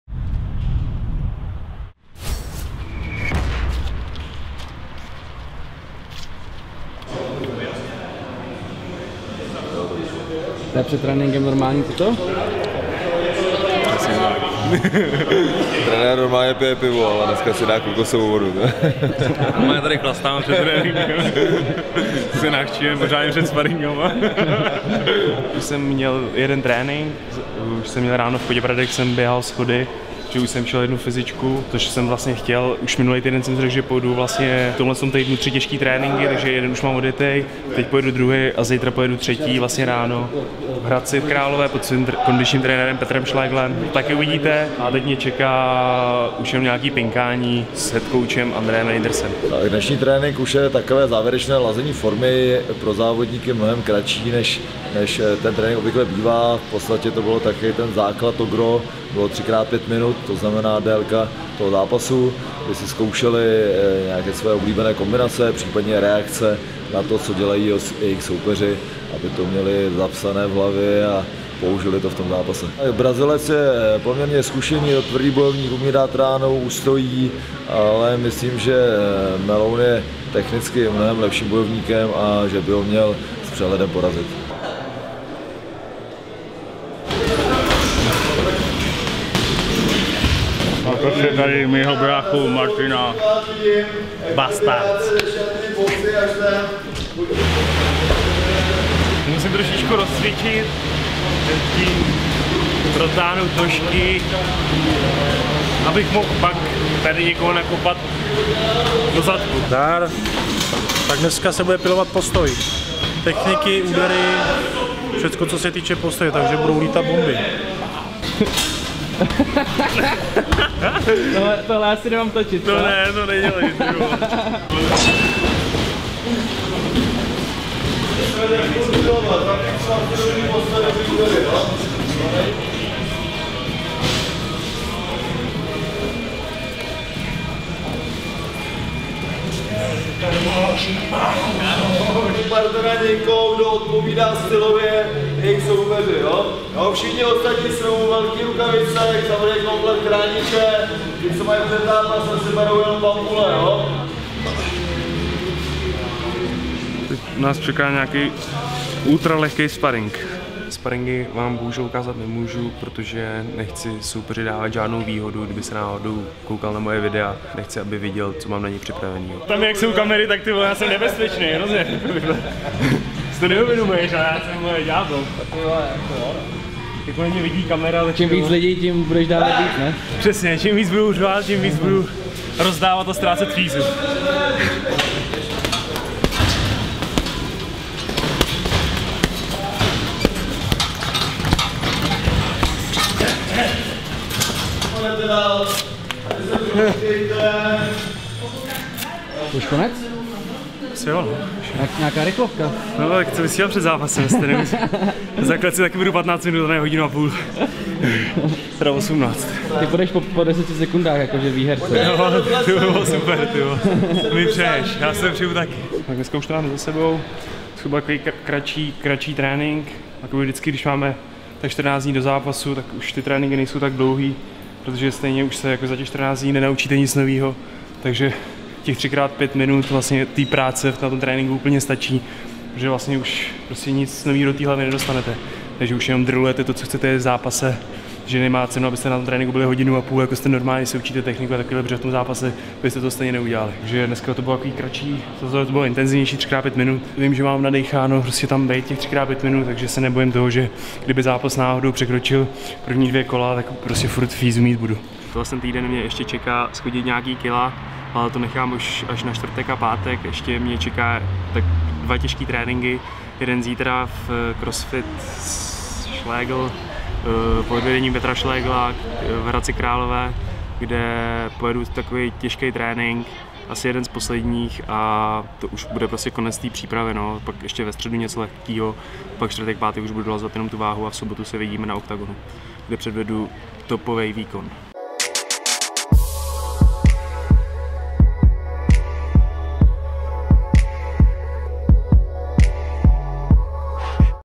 Miloš Petrášek - Cesta na OKTAGON 7 část 1.
Populární Miloš Petrášek (10-3) má znovu namířeno do OKTAGONU. Na pražské Štvanici vyzve brazilského Kovboje Alvese.